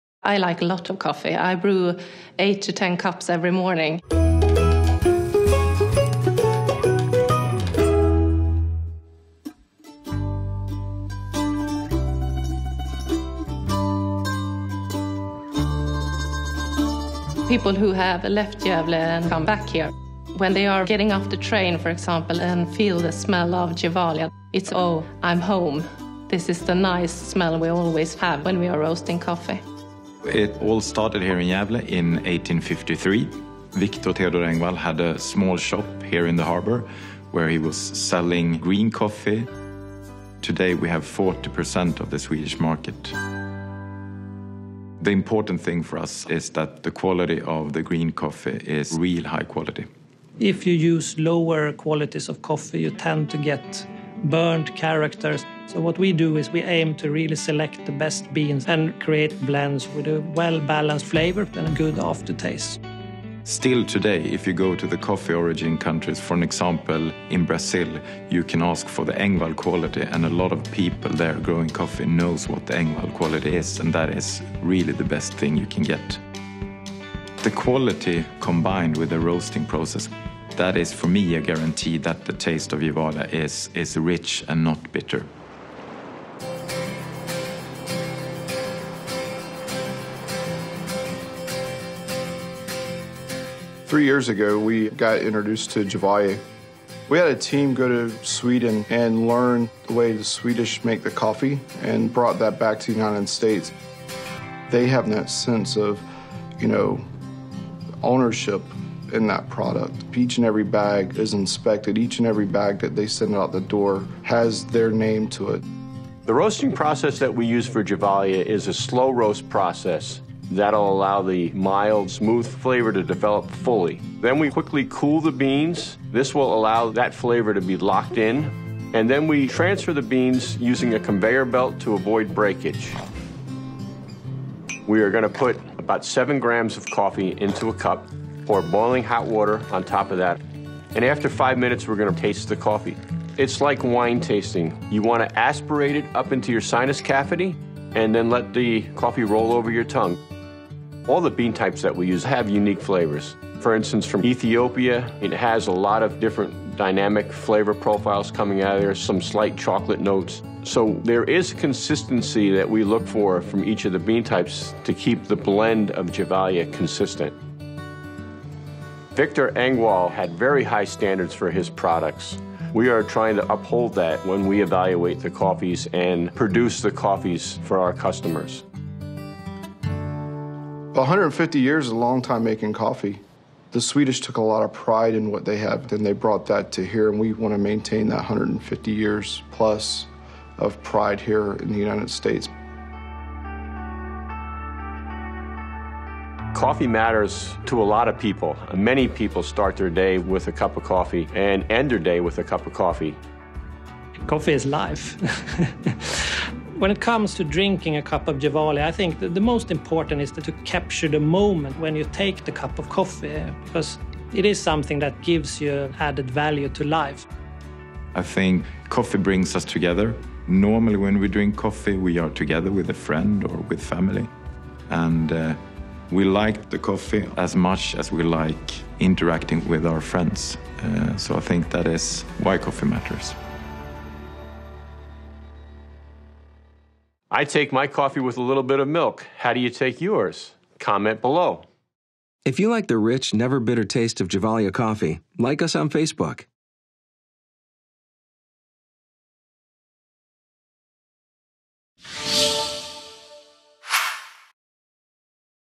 Gevalia: Coffee-Making with 150 Years of Swedish Expertise–Artisinal Food Masters–Epicuriousity
When Victor Engwall founded his coffee company in 1853, he began a tradition of Swedish coffee-making that prides itself on upholding the highest quality from beans to brew. Today, Gevalia Kaffe continues what Engwall started with expert coffee-makers around the globe who focus on producing a consistent coffee experience that’s always rich, never bitter.